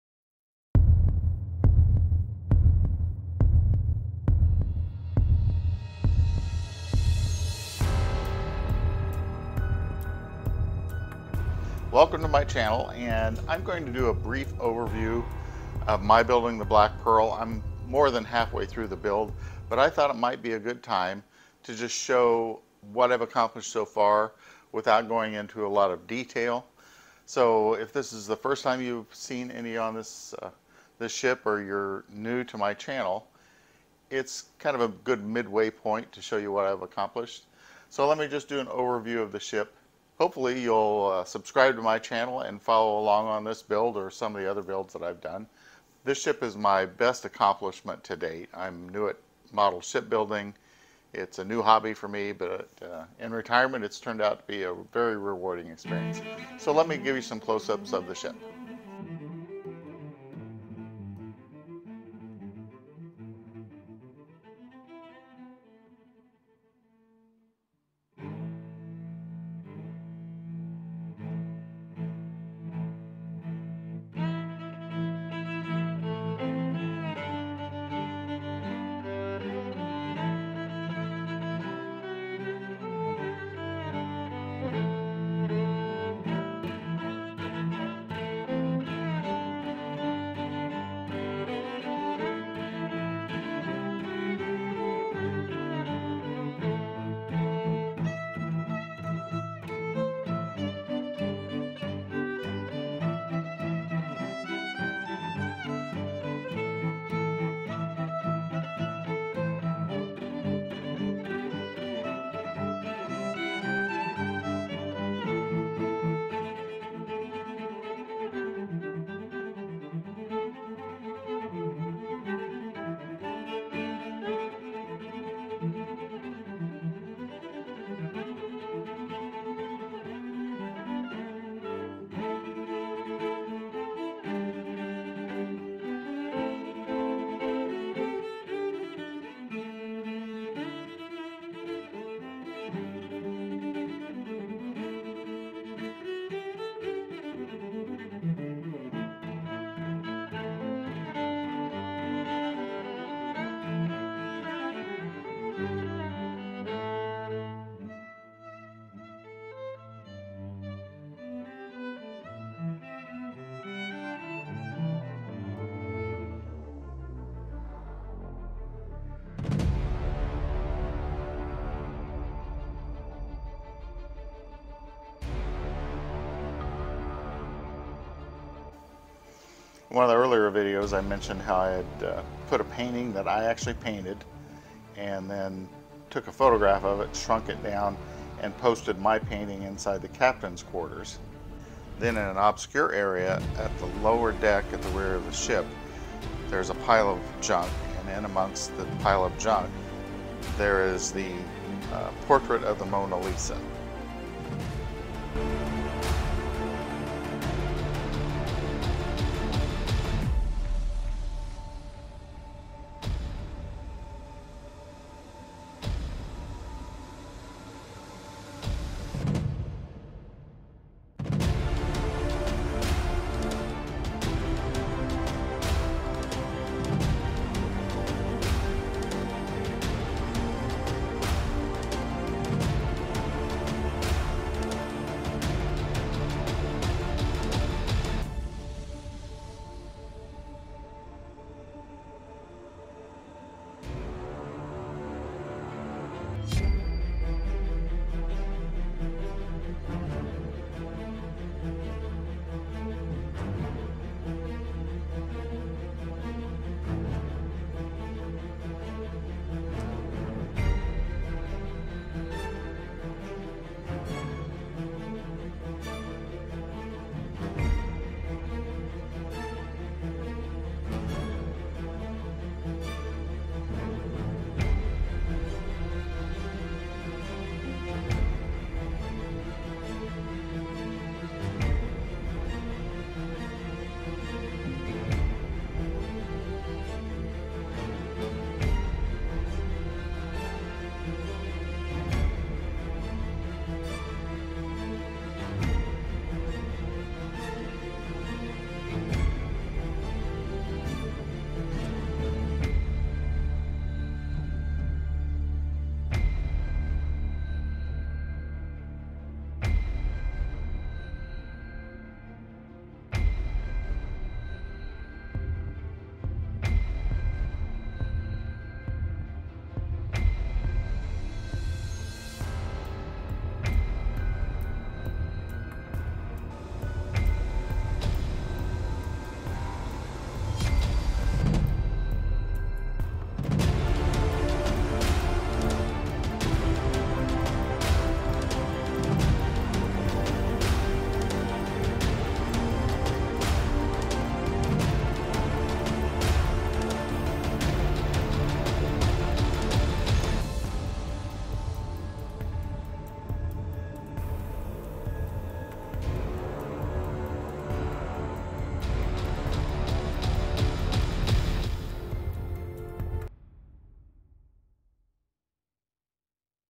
Black Pearl All Scenario Version PART 18 - Mid-Build SPECIAL
Visual review of the building of The Black Pearl - All Scenario Version